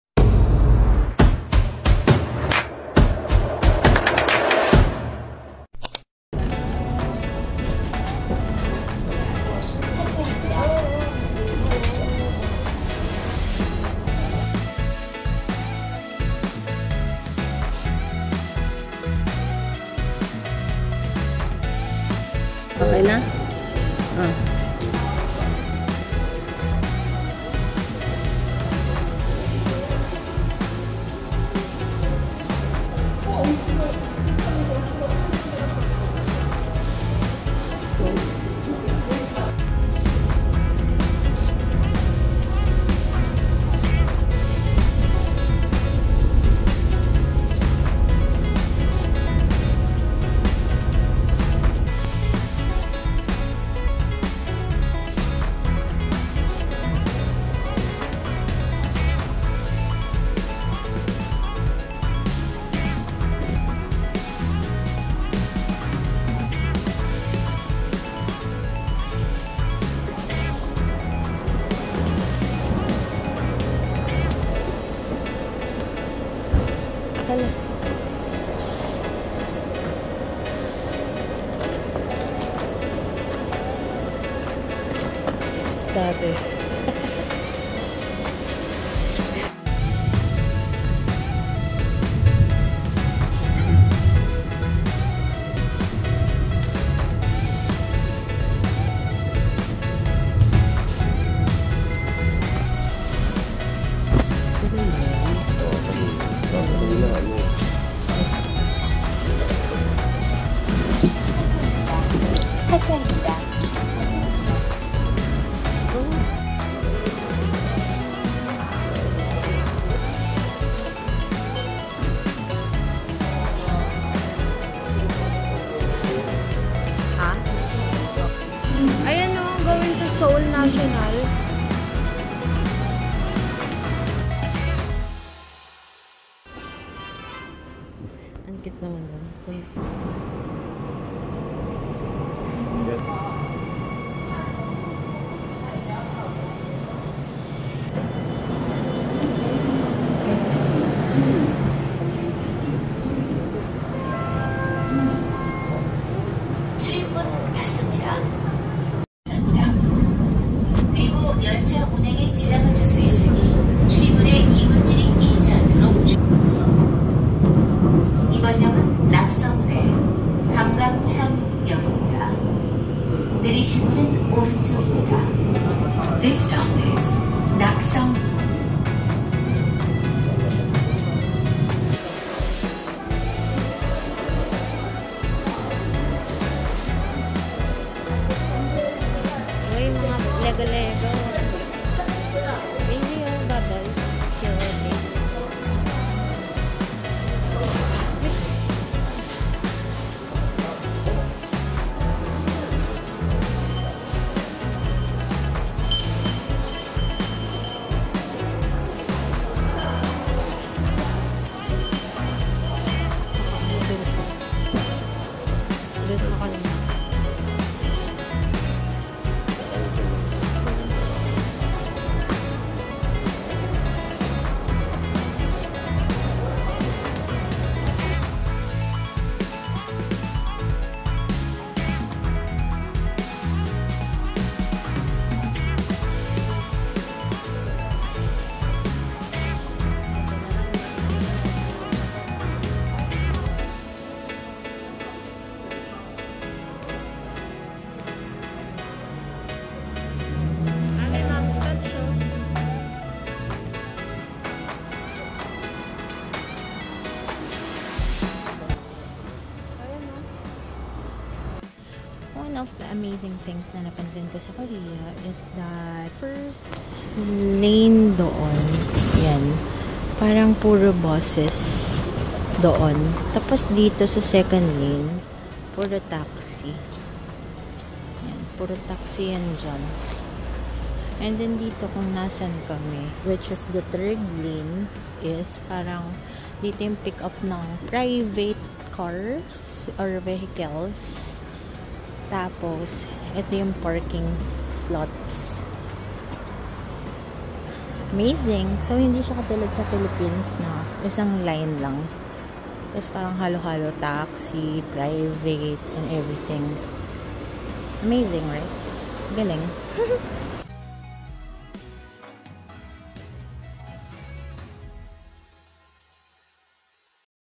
Korea's Mass transport System
Exploring the vibrant streets of South Korea is a breeze thanks to its incredibly efficient mass transport system! 🌟 From subways to buses, getting around is not only convenient but also a chance to experience the local culture. 🚉✨. So hop on and start your adventure without any hassle! 🇰🇷Hoping to be back to explore more of your beauty SK! ❤️ TravelMadeEasy DiscoverKorea TaraKoreaNa TaraKoreaNaPVCKorea Tourism Organization Manila Office